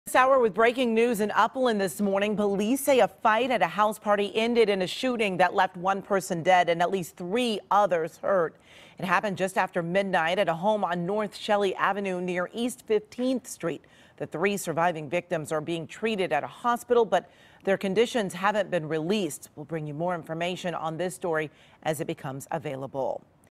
Gunfire Erupts At House Party In Upland Leaving 1 Dead, 3 Injured
Upland police say officers responded to a home in the 1500 block of North Shelly Avenue near East 15th Street where a shooting had occurred just after midnight. Amy Johnson reports.